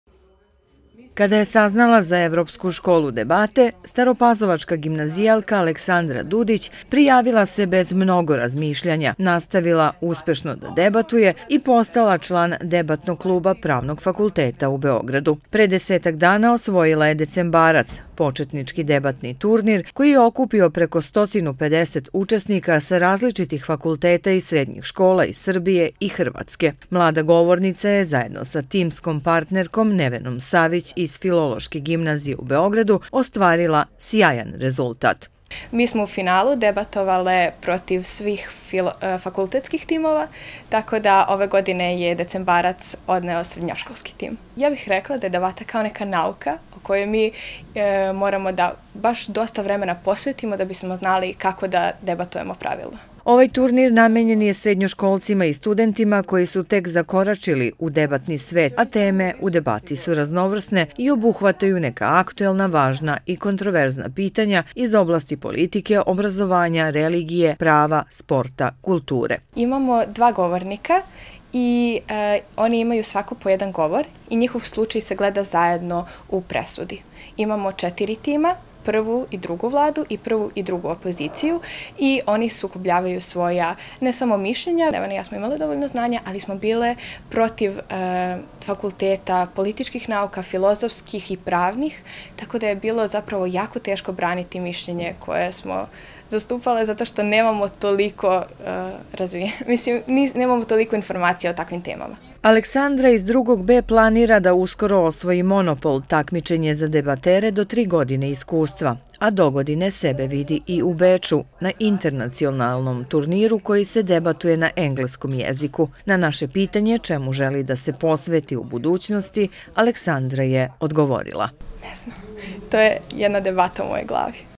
Gimnazijalka Aleksandra Dudić - najbolji debater 2022
Почетнички дебатни турнир „Децембарац“ се већ више од 20 година одржава на Правном факултету у Београду и окупља дебатере из Србије и региона.Овогодишња победница Децембарца је Александра Дудић из 2Б Гимназије Бранко Радичевић из Стара Пазове.